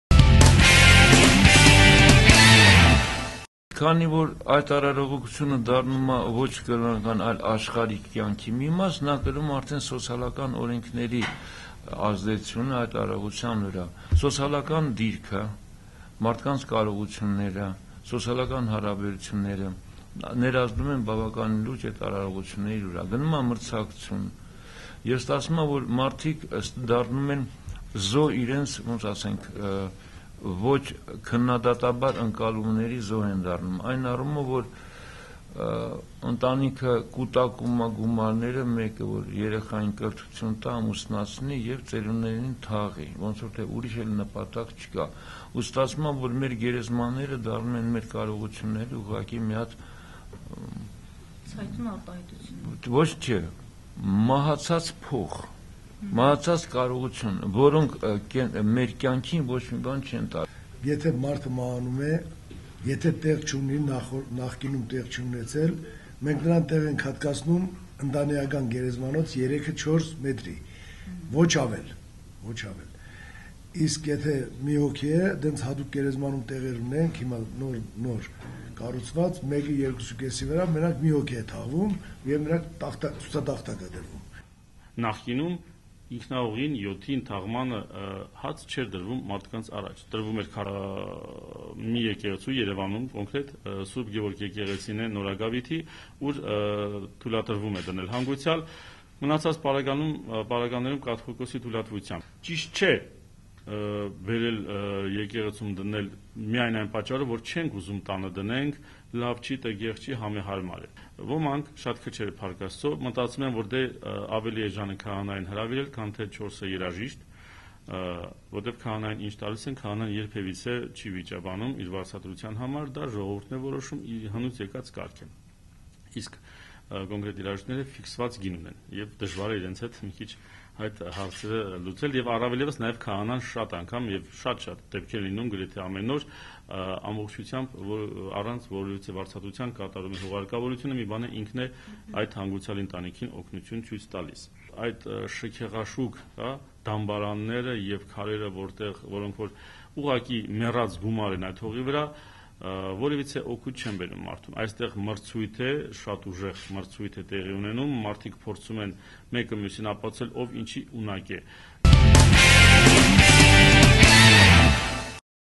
slaq.am «Թաղման արարողակարգը վերածվել է ցուցադրականի՞»
Հայաստանում թաղման արարողությունը դարձել է ոչ թե կրոնական այլ աշխարհիկ կյանքի մի մաս և կրում է սոցիալական բնույթ: Ստացվում է, որ մարդիկ դառնում են քննադատակամն ընկալումների զոհ և մեր գերեզմանները դառնում են «մահացած փող», «մահացած կարողություն», որոնք մեր կյանքին ոչինչ չեն տալիս: Այսօր «Դե Ֆակտո» մամուլի ակումբում կայացած ասուլիսում, խոսելով Հայաստանում թաղման արարողությունների մասին, ասաց սոցիոլոգ Ահարոն Ադիբեկյանը: Ահարոն Ադիբեկյանը նշեց, որ հայերի մոտ ճոխ գերեզմանները ինքնահաստատման ձև է: